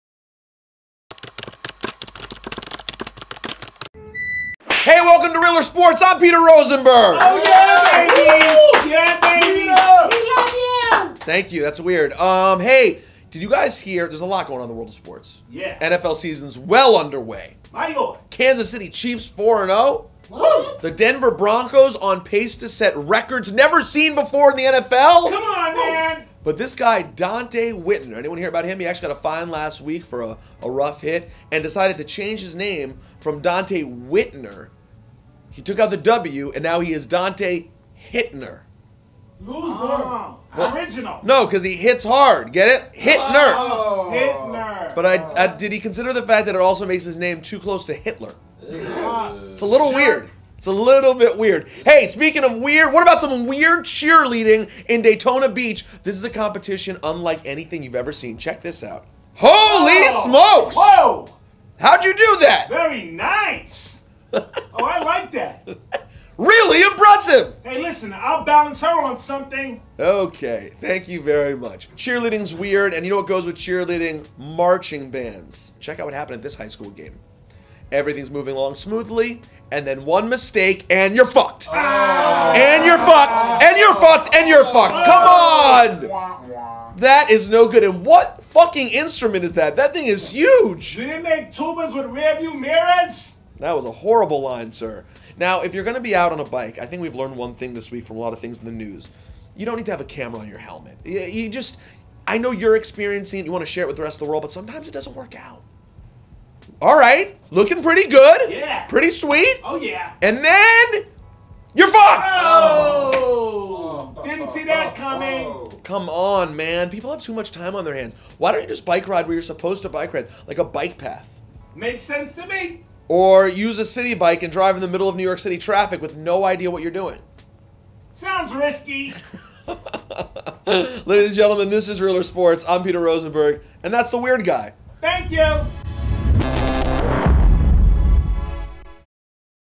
REALER SPORTS - Ep31 - Hot Cheerleaders and Terrible Bikers
What could go wrong when you wear a camera on your helmet while biking?  And what in the hell is human domino's?